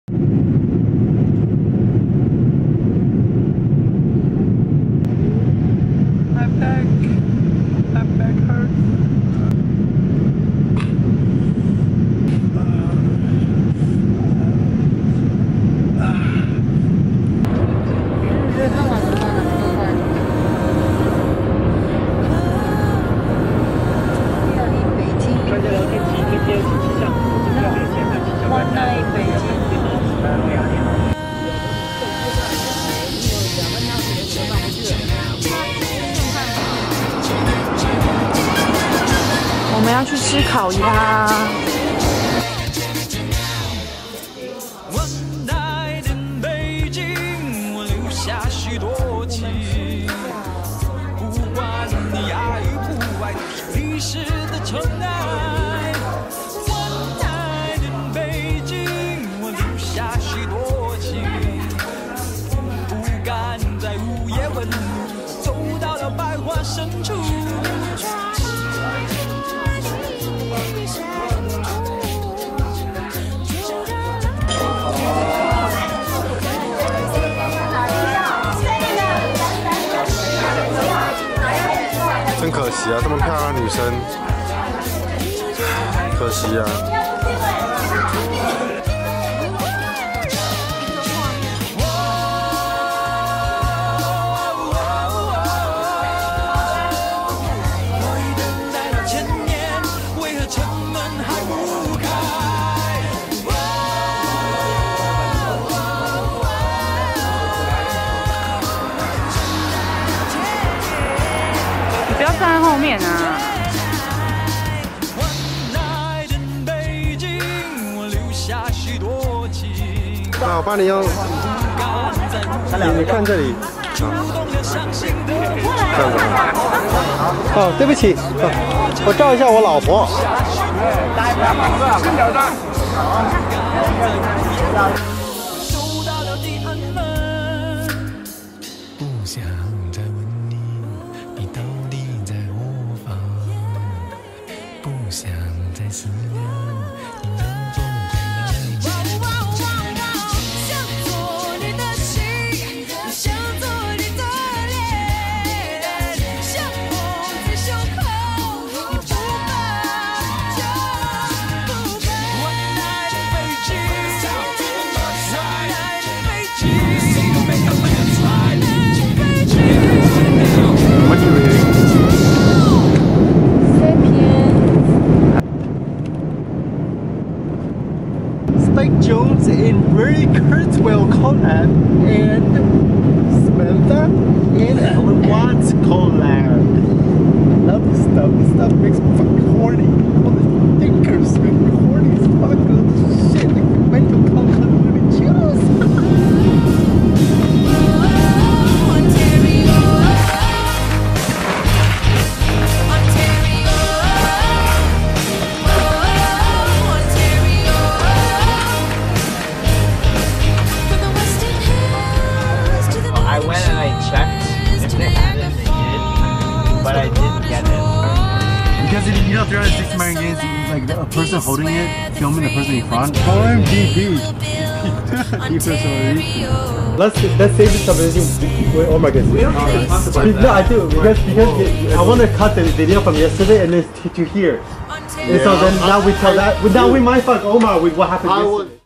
Trip to Canada - One night in Beijing
I quit my job at Hooters and began my trip to North America again
I felt so nervous about it because I got deported by US custom in YYZ in 2016.
But first, we transfered flight in Beijin for one night...